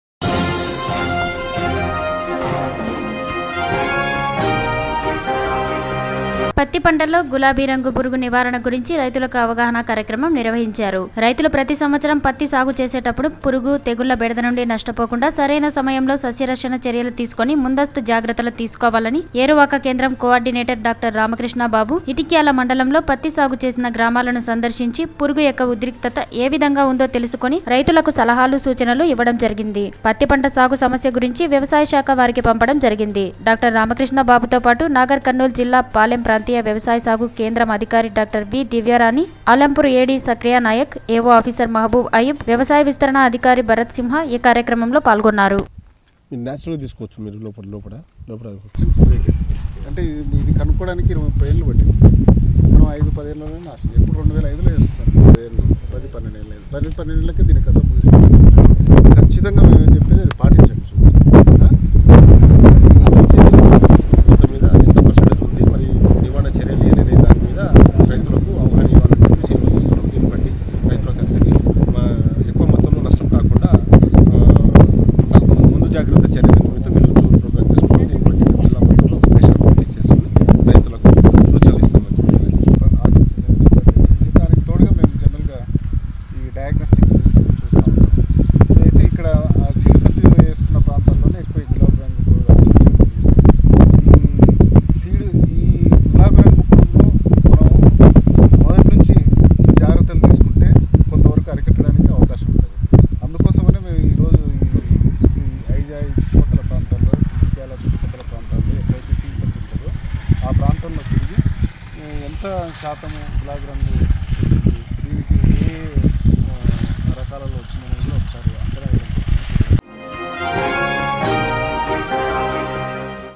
పత్తి పంటలో గులాబీ రంగు పురుగు నివారణ గురించి రైతులకు అవగాహన కార్యక్రమం
పత్తి పంటలో గులాబీ రంగు పురుగు నివారణ గురించి రైతులకు అవగాహన కార్యక్రమం.  రైతులు ప్రతి సంవత్సరం పత్తి సాగు చేసేటప్పుడు పురుగు, తెగుళ్ల బెడద నుండి నష్టపోకుండా సరైన సమయంలో సస్యరక్షణ చర్యలు తీసుకోని ముందస్తు జాగ్రత్తలు తీసుకోవాలని ఏరువాక కేంద్రం కో ఆర్డినేటర్ డా. రామకృష్ణ బాబు(శాస్త్ర వేత్త).ఇటిక్యాల మండలంలో పత్తి సాగు చేసిన గ్రామాలను సందర్శించి పురుగు యెక్క ఉద్రిక్తత ఏ విధంగా వుందో తెలుసుకుని రైతులకు సలహాలు, సూచనలు ఇవ్వడం జరిగింది పత్తి పంట సాగు సమస్య గురించి వ్యవసాయ శాఖ వారికి పంపడం జరిగింది. డా.రామకృష్ణ బాబు గారితో పాటు నాగర్ కర్నూల్ జిల్లా పాలెం .ప్రాంతీయ వ్యవసాయ కేంద్రం అధికారి డా. వి. దివ్యరాణి గారు, అలంపూర్ ఏ. డి సక్రియా నాయక్ గారు, ఏ. వో ఆఫీసర్ మహబూబ్ ఆయుబ్ గారు, వ్యవసాయ విస్తరణ అధికారి భరథ్ సింహ గారు పాల్గొన్నారు...